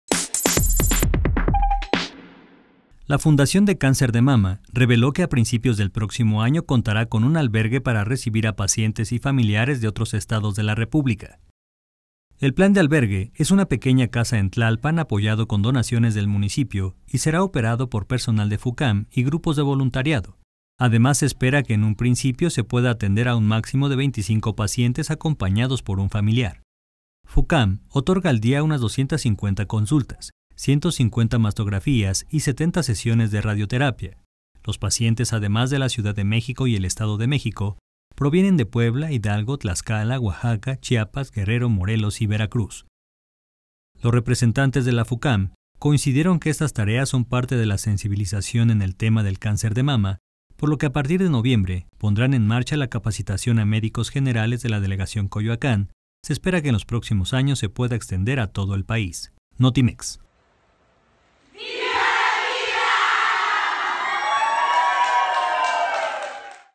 Fundación de Cáncer de Mama contará con albergue para los pacientes
Fucam otorga al día 250 consultas, 150 mastografías y 70 sesiones de radioterapia.